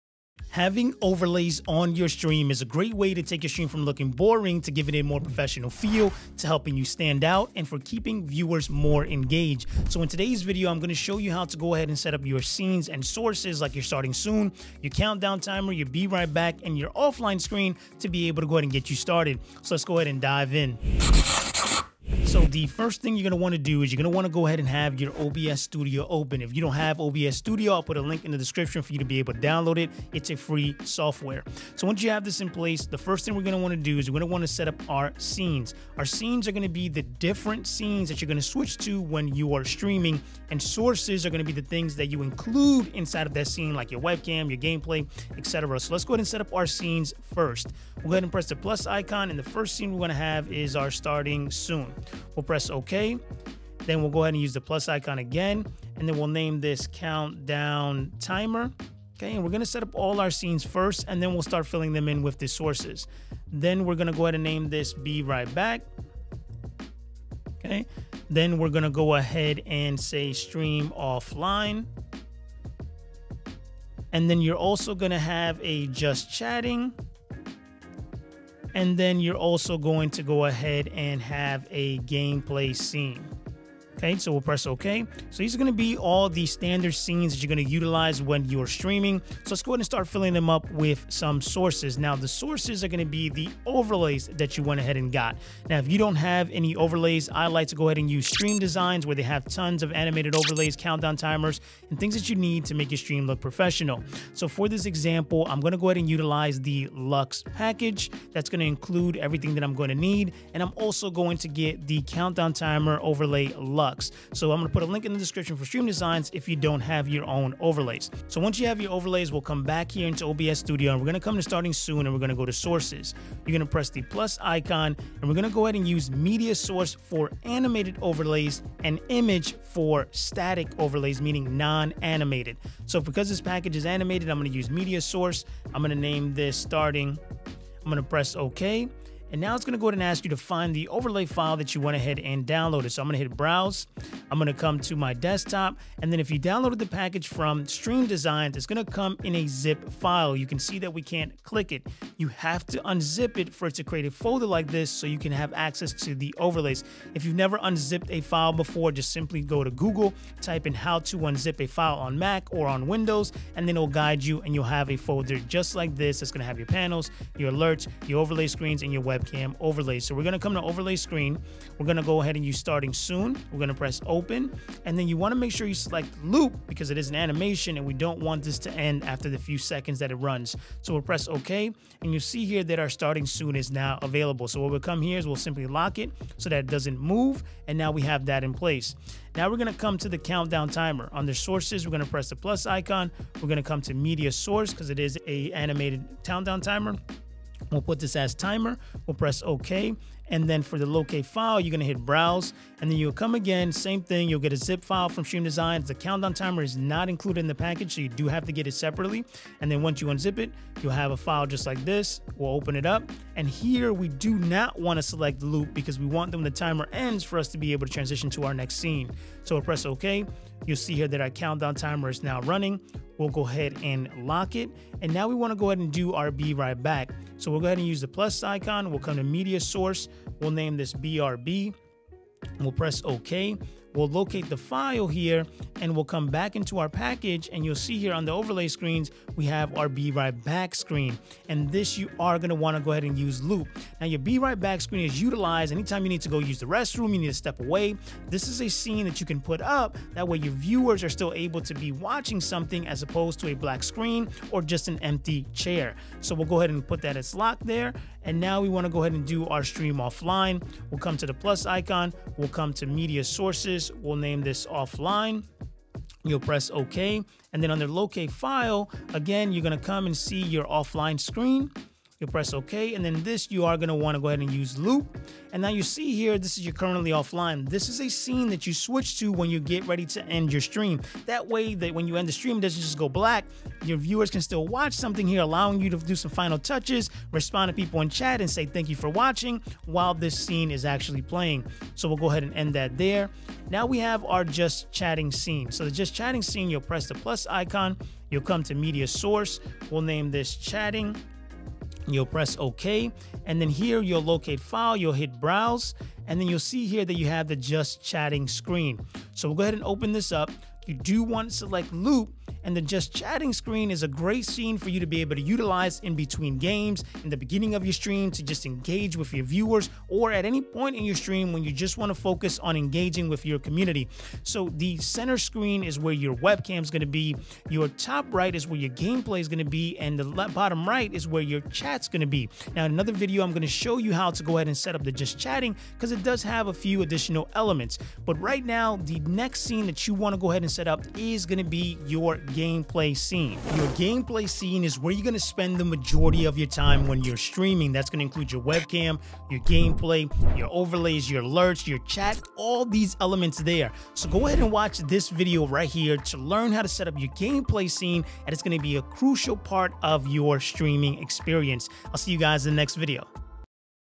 How to Setup Overlays in OBS Studio for Beginners
In this video, I'll show you how to set up scenes, sources, and overlays in OBS.

Watch this video next:
👉 OBS Studio - How to Add Game, Webcam & Overlays

🛑 Gear I Use to Stream & Make Content 👇

My Pro Streaming Setup:

About - This video will walk you through the steps needed to set up scenes, sources, and overlays in OBS.